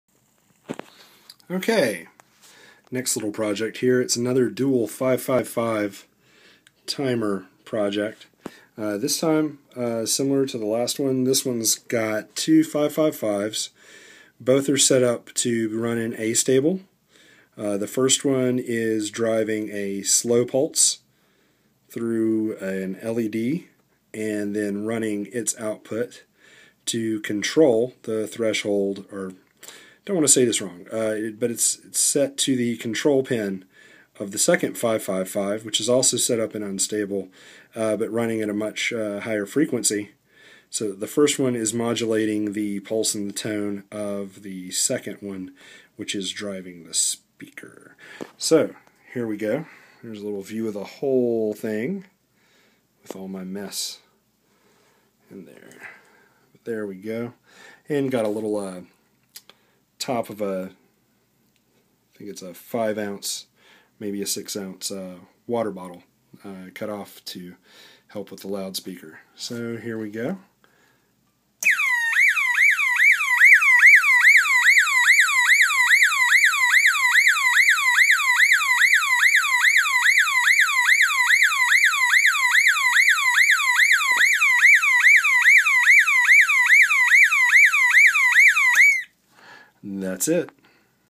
555 timer alarm
From the book: "Make: Electronics" Experiment 17, version from Fig 4:30.
Alteration from book's schematic: C4 is 220uf for a longer sweep.